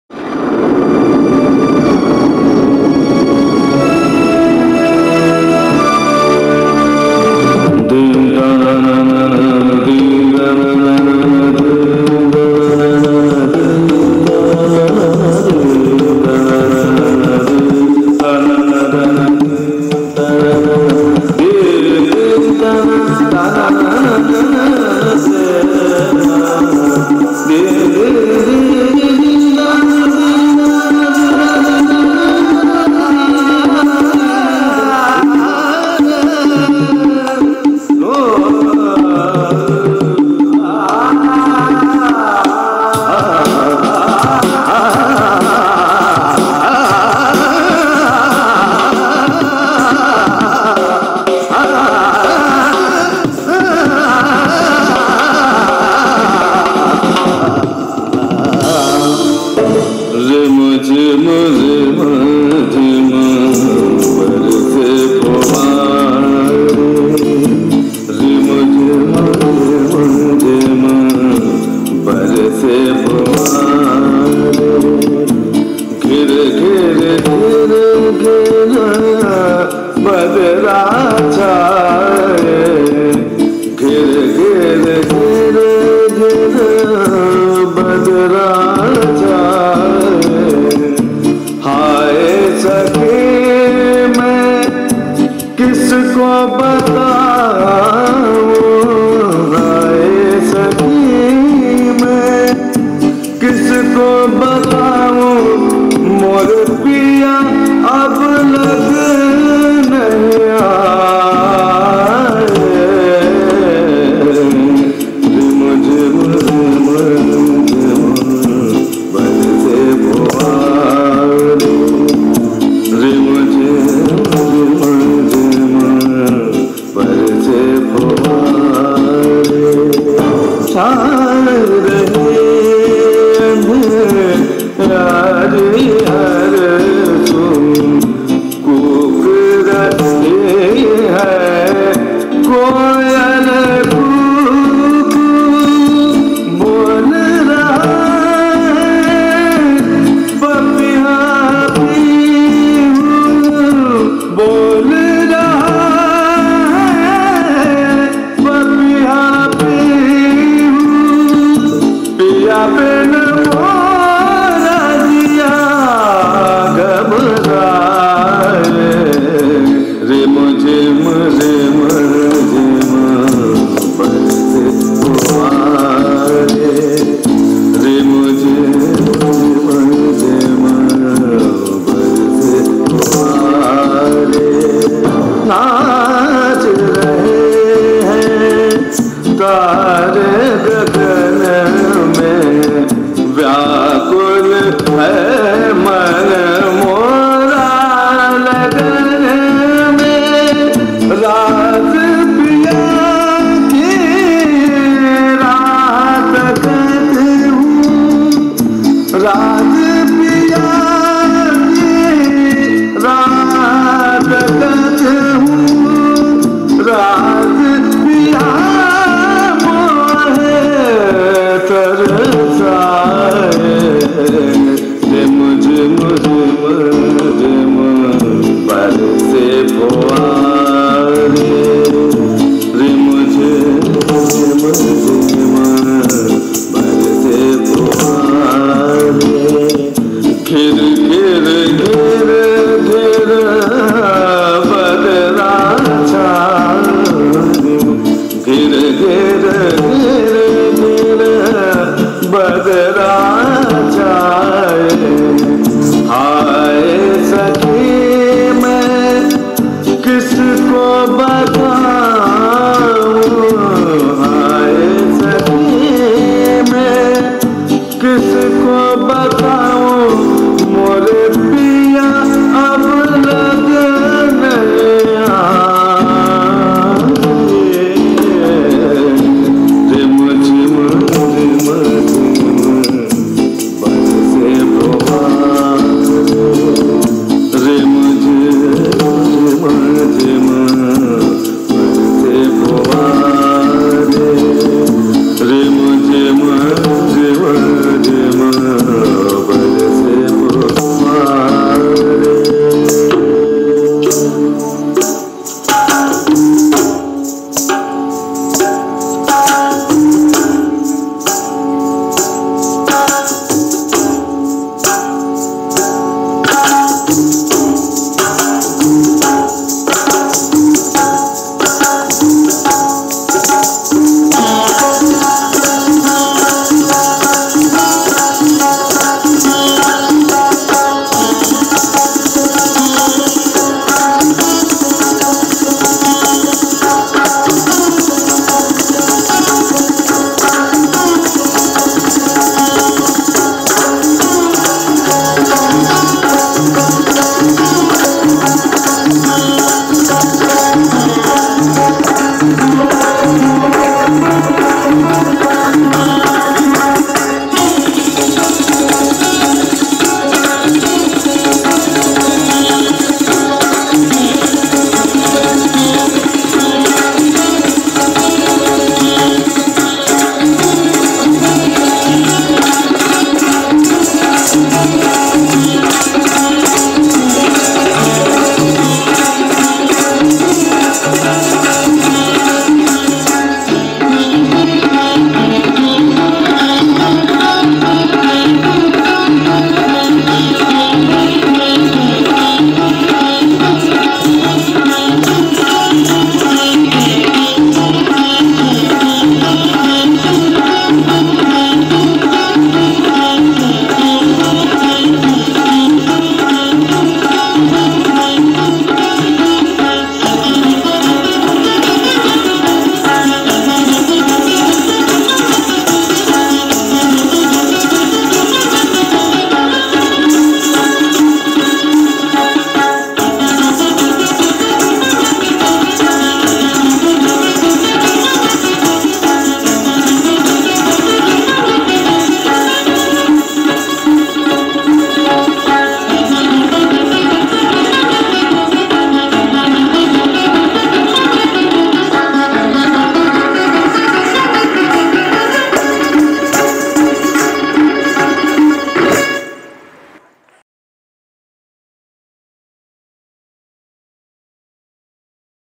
Kathak by Souvik Chakraborty | EZCC
@ezcckolkata, @MinistryofCultureGovtofIndia in collaboration with Kuchupudi Kalamadhuri and @ghungroodanceacademy presented a Solo Kathak Performance by Shri @souvikchakraborty516